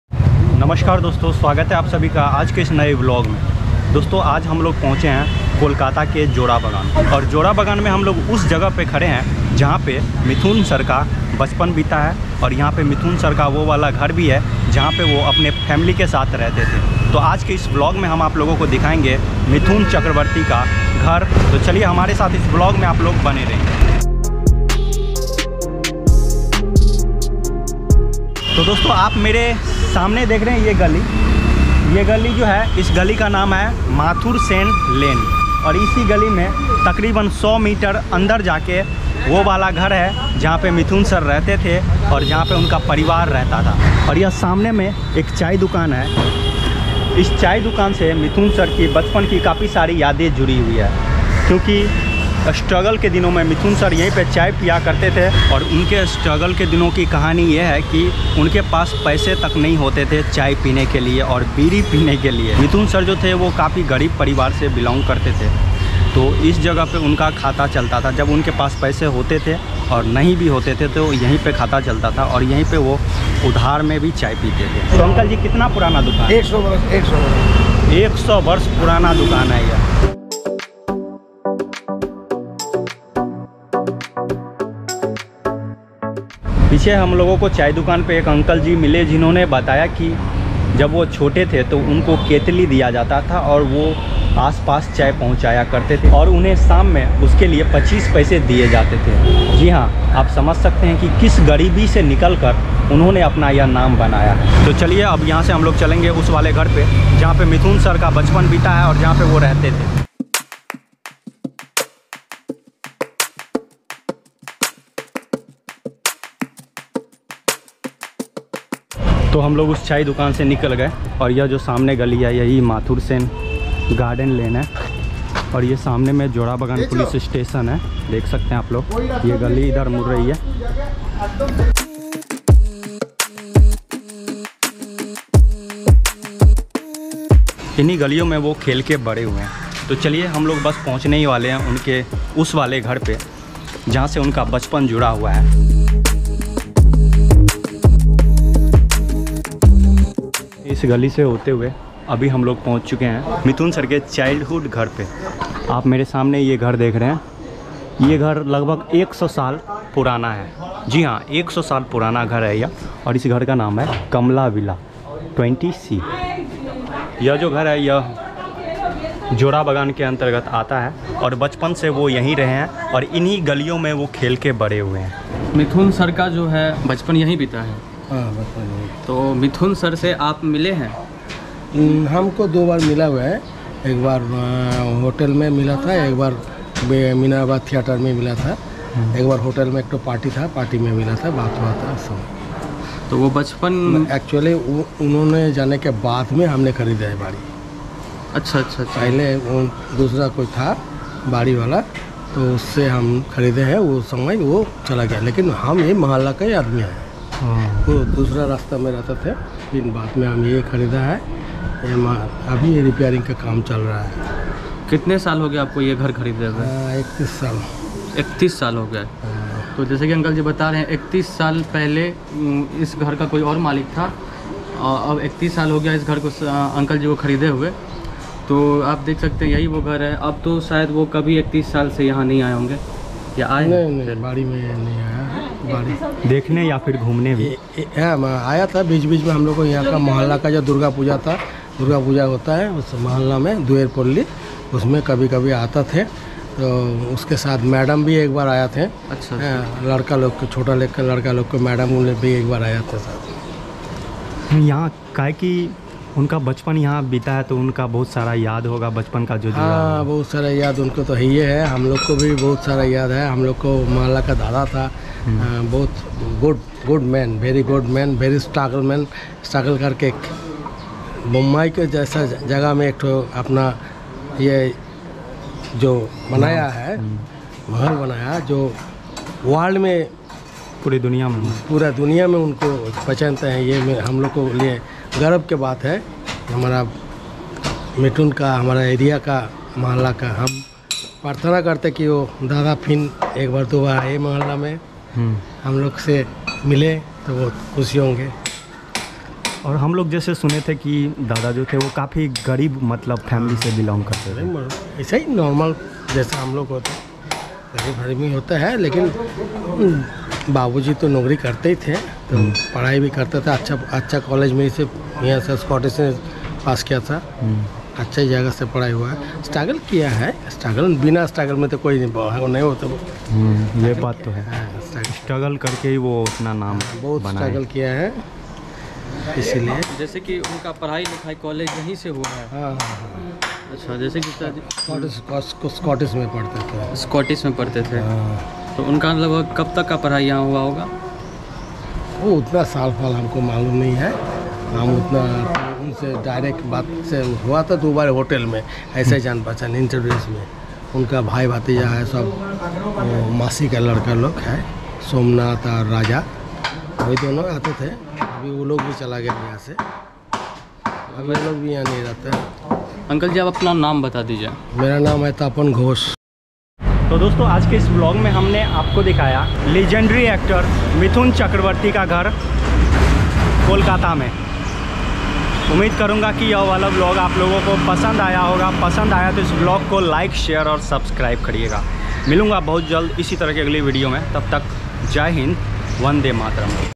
मिथुन चक्रवर्ती का घर कोलकाता || Mithun chakraborty ka ghar kolkata || skj vlogs
Mithun chakraborty House kolkata || मिथुन चक्रवर्ती का घर कोलकाता || skj vlogs

Aaz ke is vlog me mai aapko leke jaane wala hu bollywood ke famous actor mithun chakraborty ke ghar, jinka janm kolkata me hua hai. Aur kolkata ke jorabagan me unka ghar hai jaha pe unke family rehte the aur jaha unka bachpan bita hai.